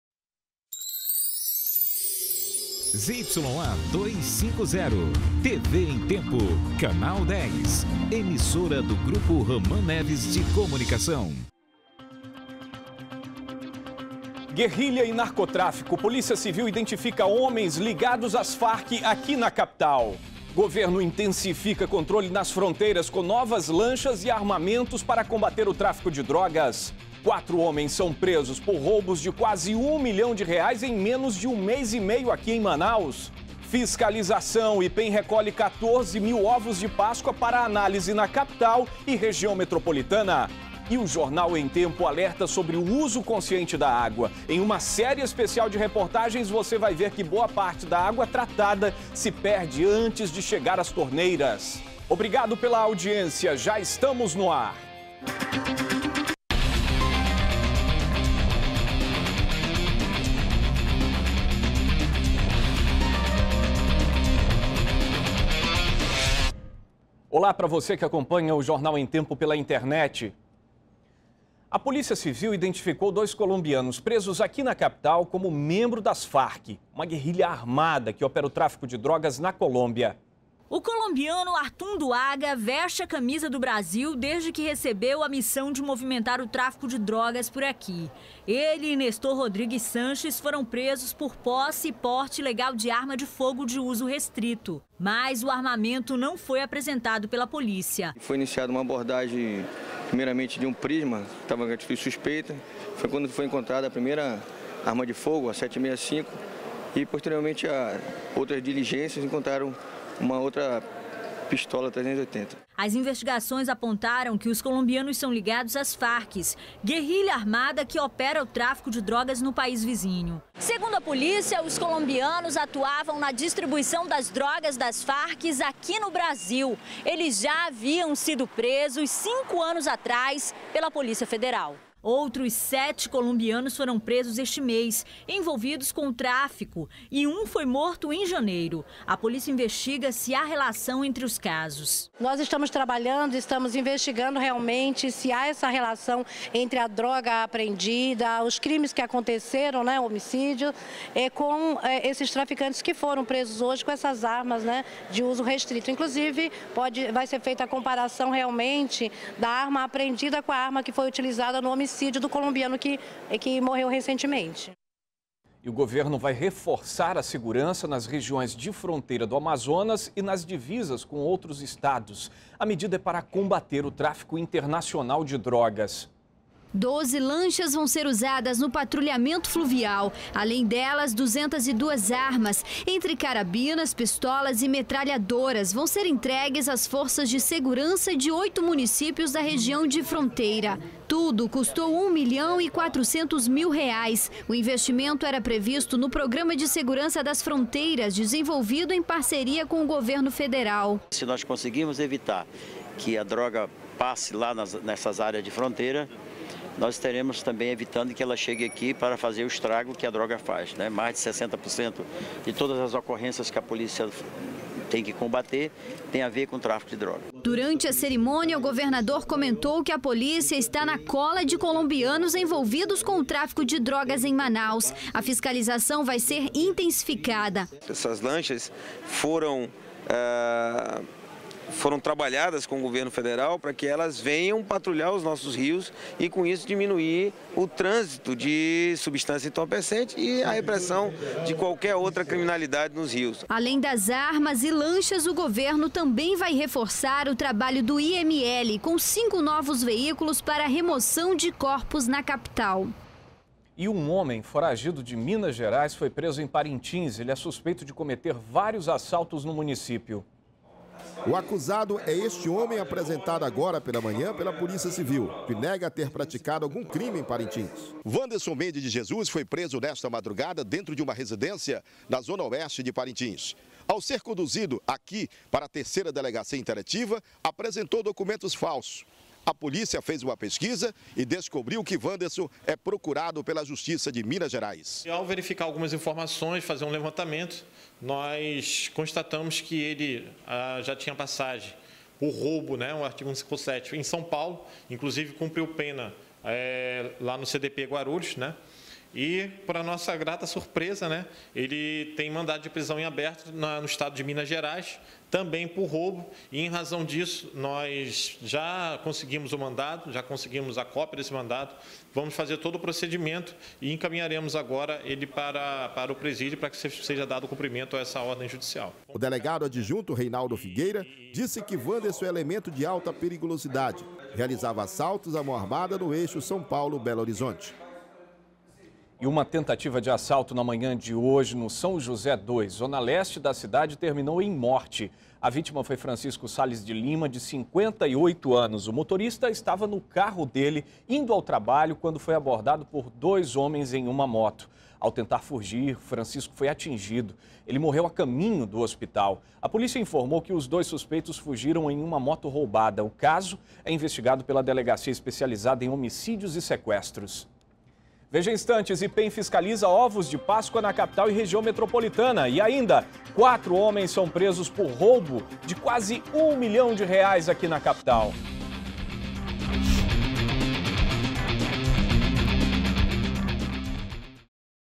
Governo reforça segurança nas fronteiras com novas lanchas e armamentos
01º BLOCO JORNAL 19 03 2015 - Governo reforça segurança nas fronteiras com novas lanchas e armamentos